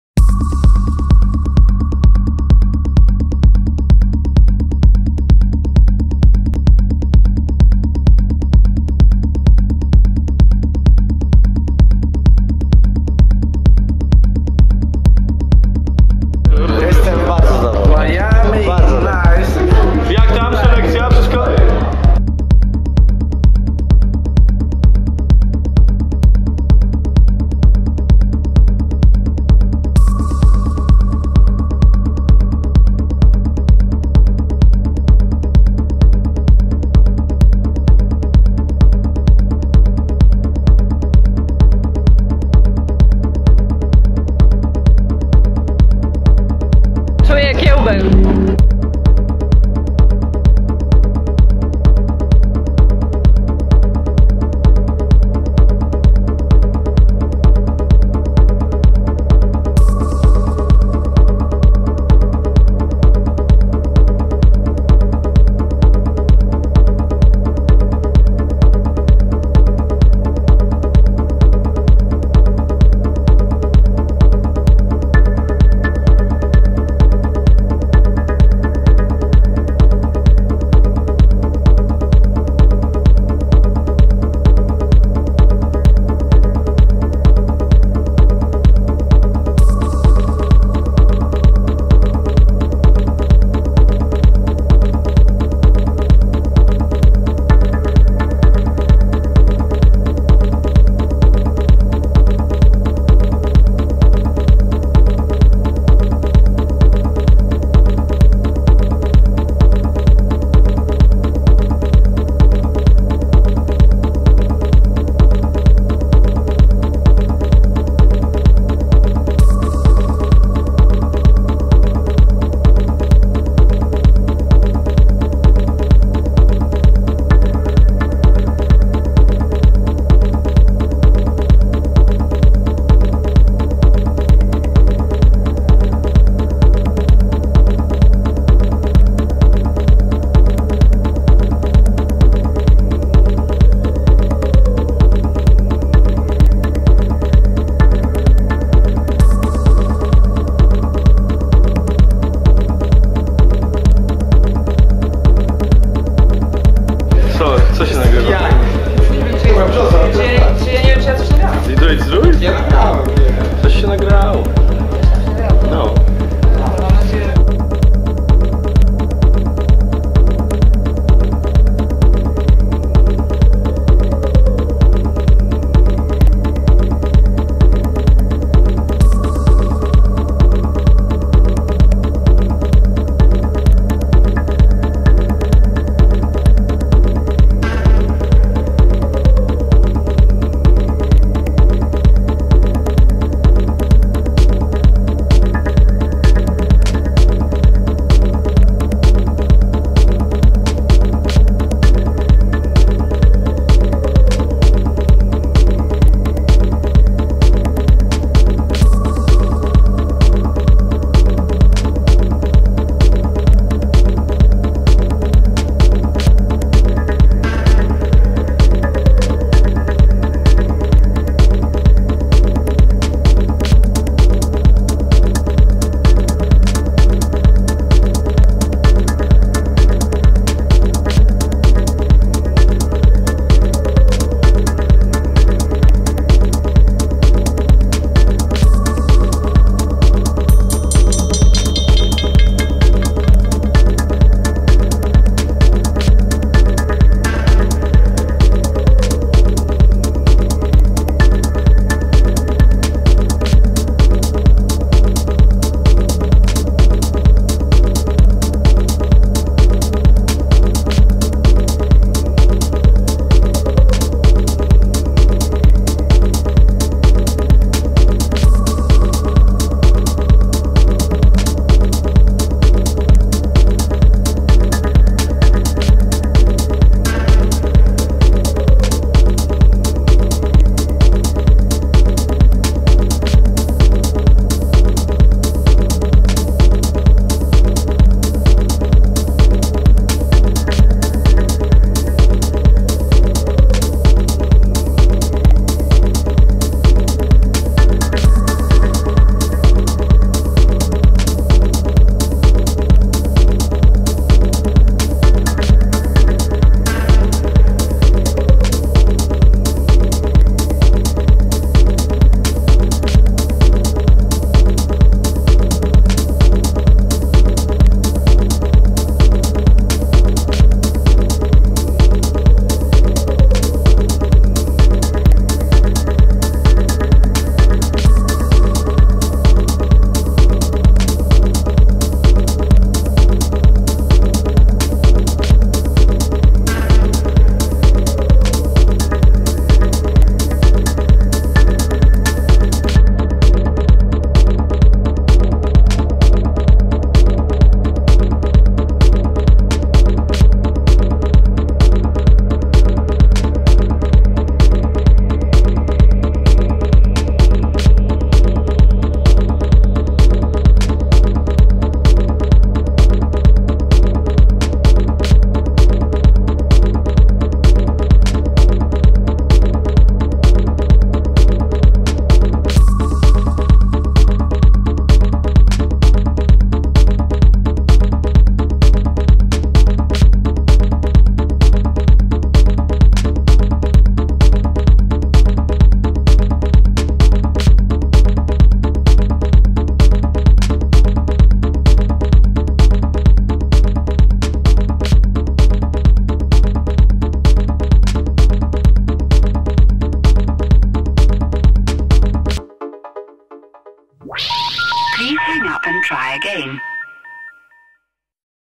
detroitZDRoJ! springbreak pres Butane
On the 12th of april we had the honour to host Alphahuse Records mastermind - BUTANE + special guest from Stockholm - Marcus Stork. Both showed marvellous skills and the party exploded. Supported as usual by eli, dip & bshosa and the unbelievable VJ work of Pani K.
detroitZDRoJ
join & support our mission